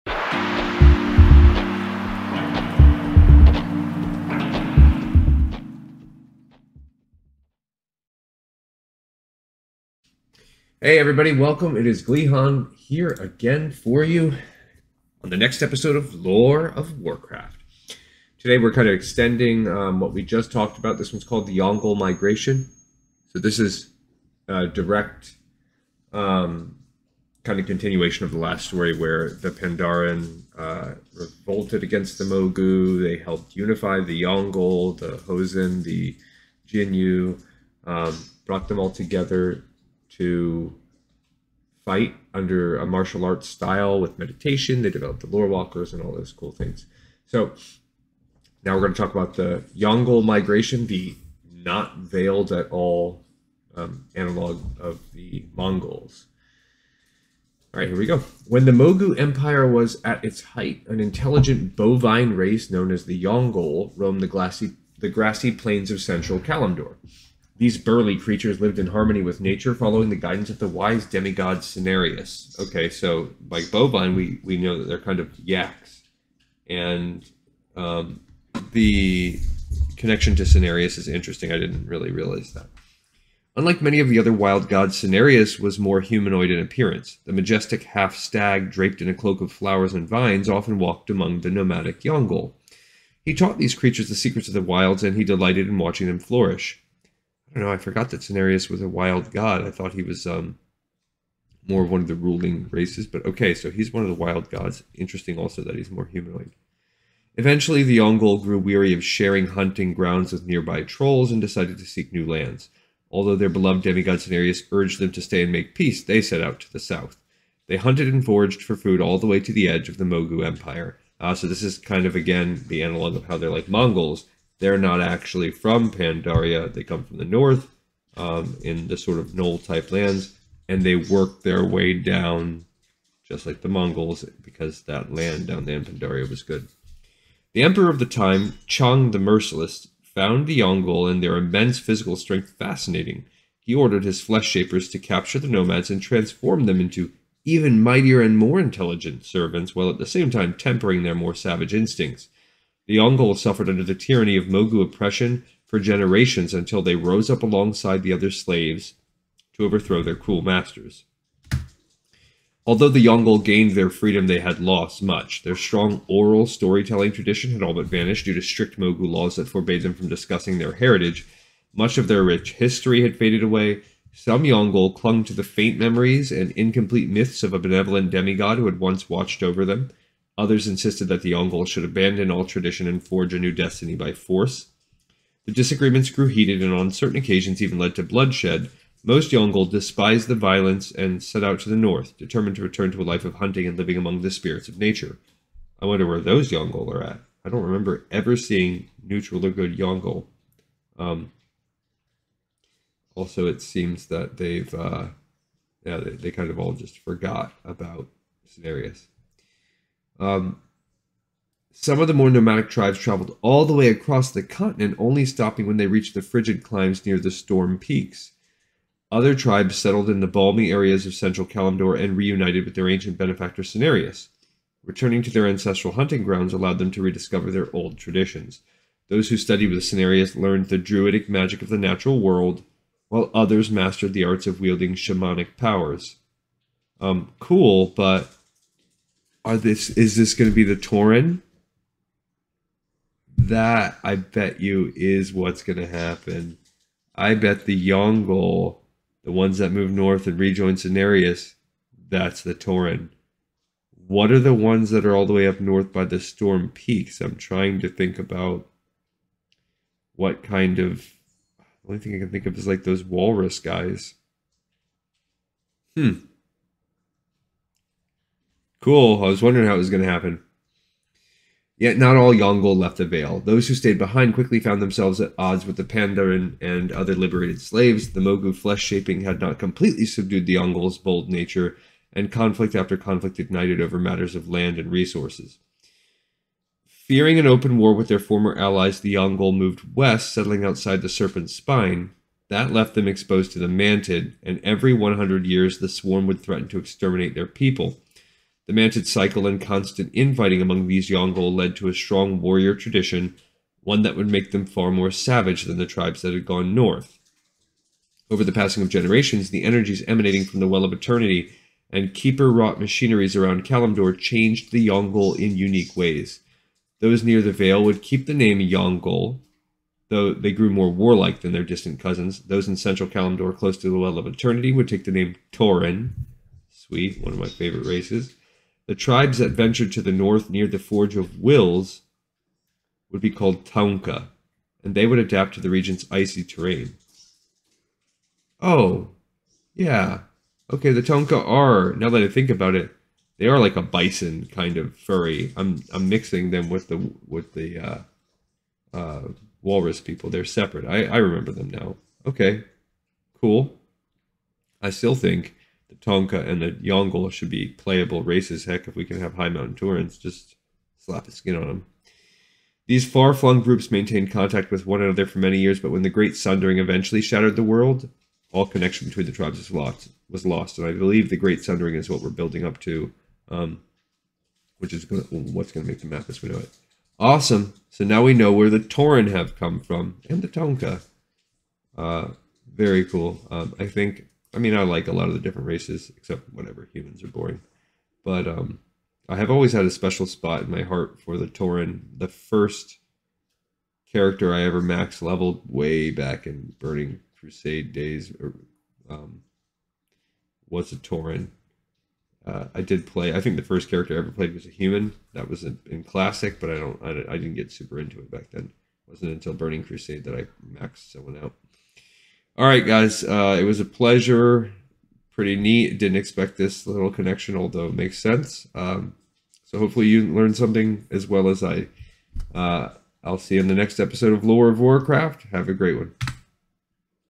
Lore of Warcraft - Episode 35 - Yaungol Migration
Chronicle Volume 1 Chapter 3 on the yaungol migration throughout Azeroth, resulting in the taunka and tauren races